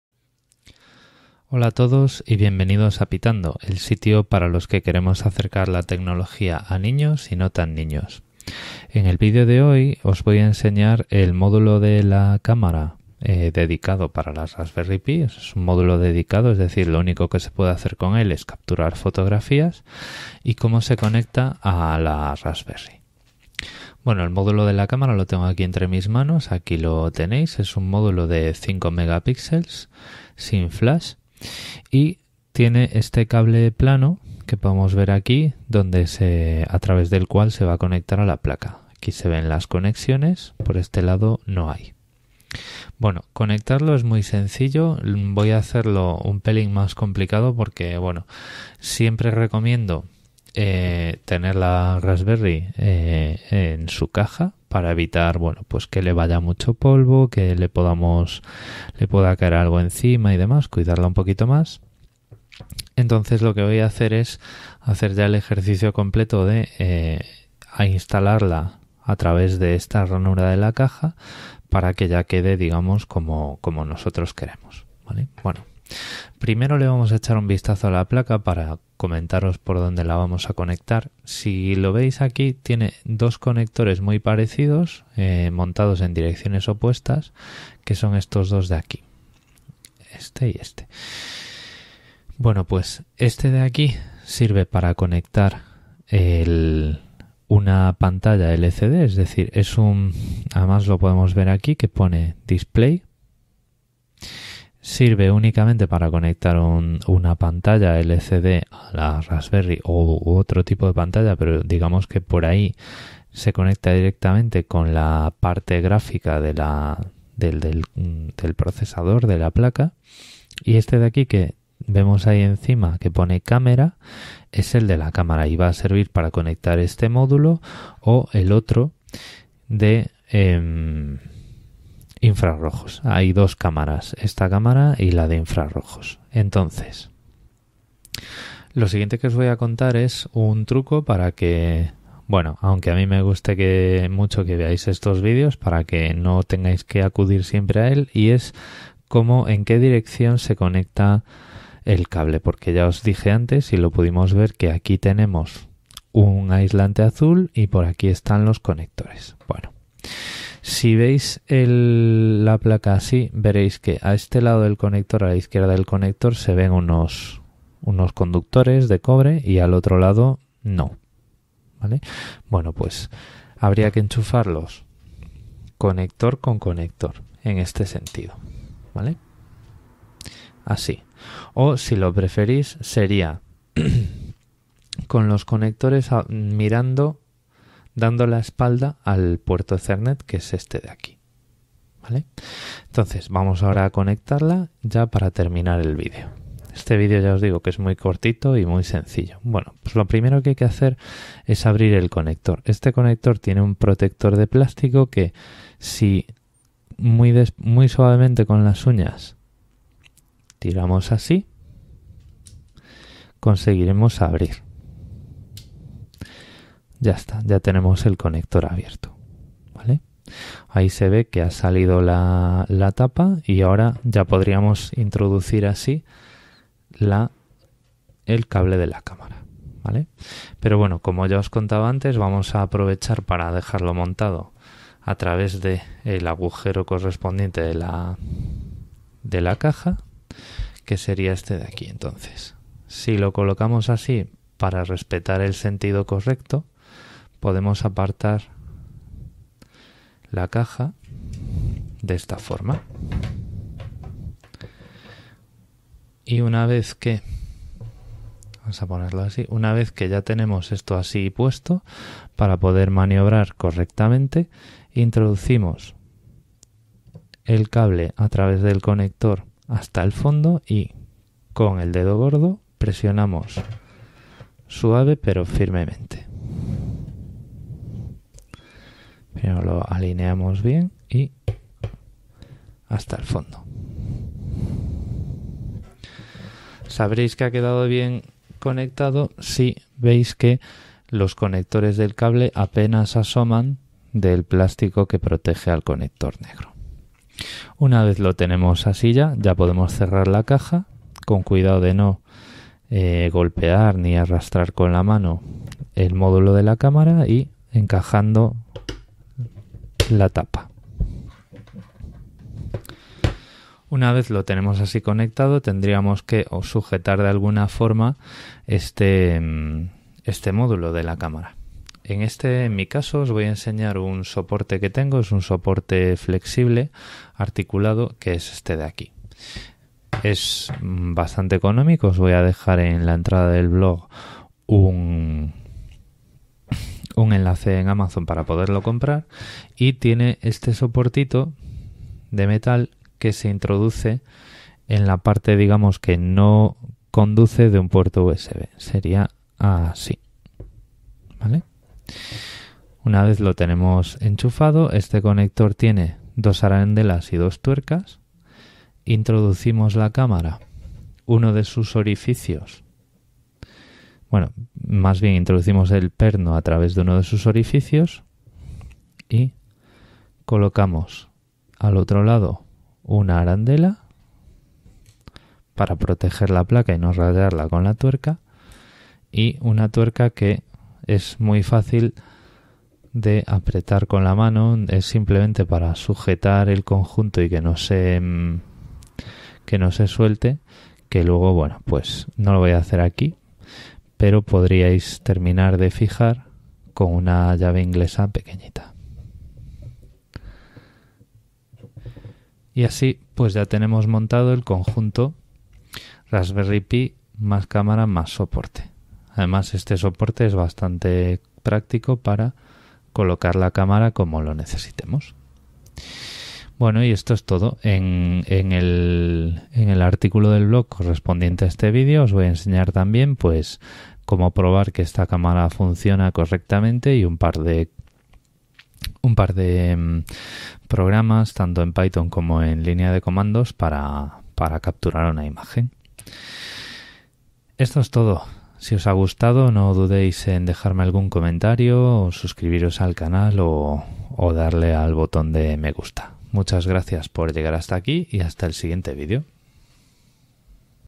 Módulo cámara para Raspberry Pi
En este vídeo os muestro cómo montar la cámara de la Raspberry Pi usando además una caja y un soporte de fijación.